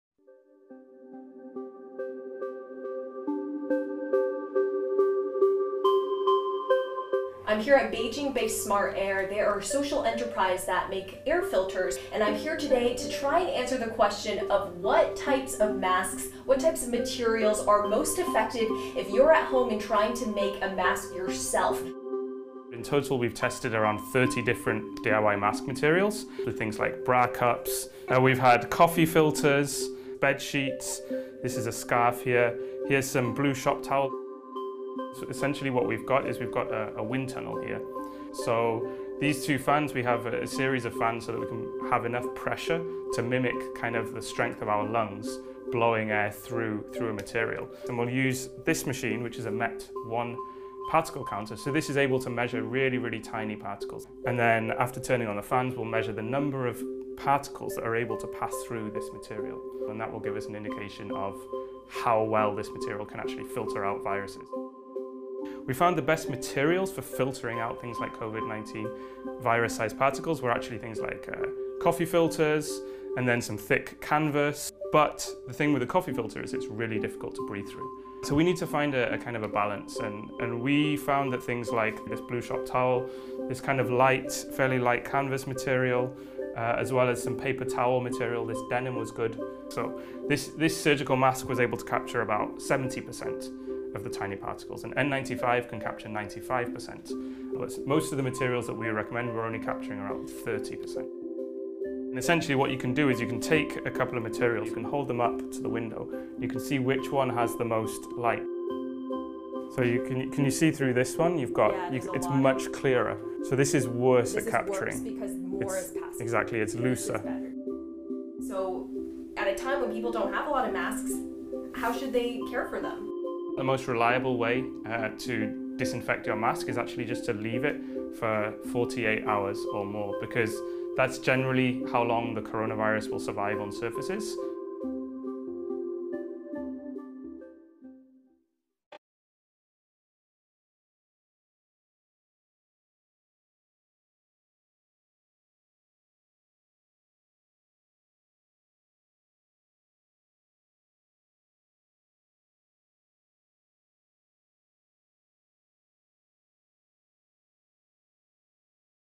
Do DIY Face Masks Actually Work?
Smart Air has tested 30 different materials to find which one filters out the most air particles similar to Covid-19.